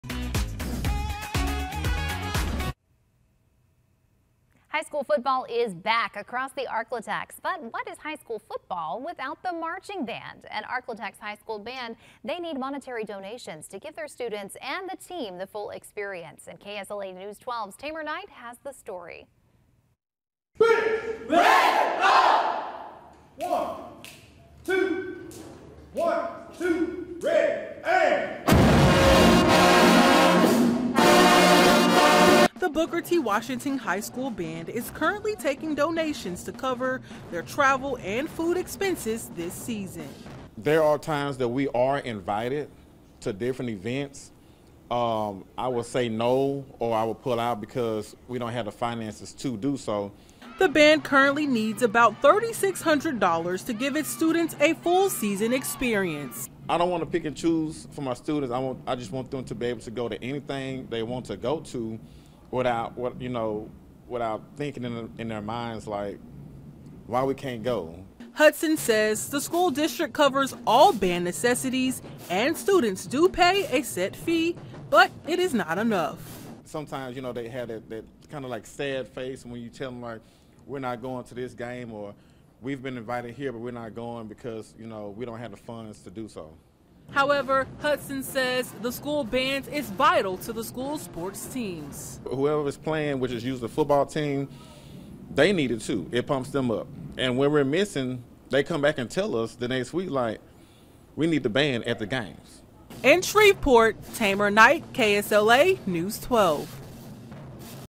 Booker T. Washington band in need of donations
The band is looking to cover a $3,600 gap in funding, so they’re reaching out to the community for help.